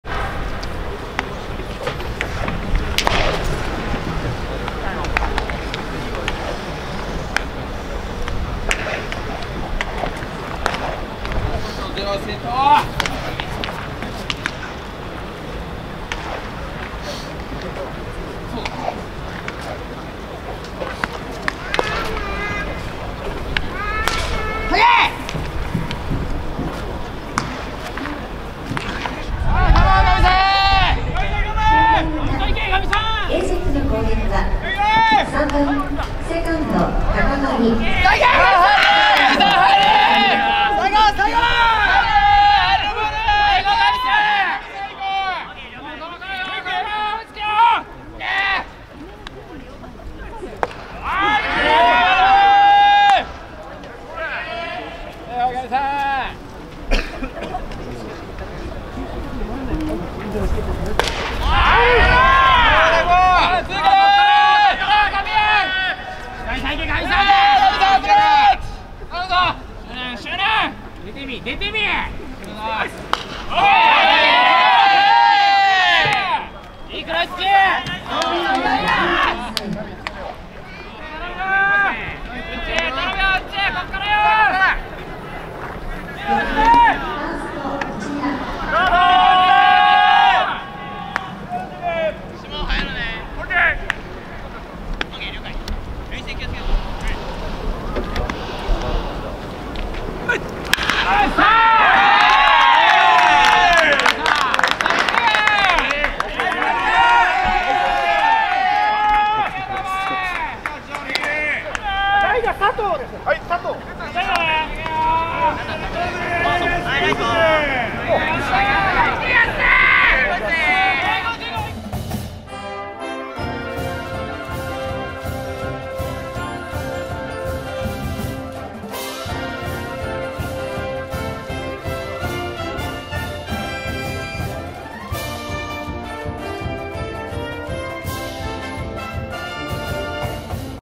【社会人野球】エイジェック新加入 元東北楽天ゴールデンイーグルス 内田靖人 初の公式戦「4番・一塁」4打数2安打の活躍 第77回JABA東京スポニチ大会 2023/3/6（月） 明治神宮野球場
【社会人野球】エイジェック新加入　元東北楽天ゴールデンイーグルス　内田靖人　「4番・一塁」4打数2安打の活躍　第77回JABA東京スポニチ大会 2023/3/6（月） 明治神宮野球場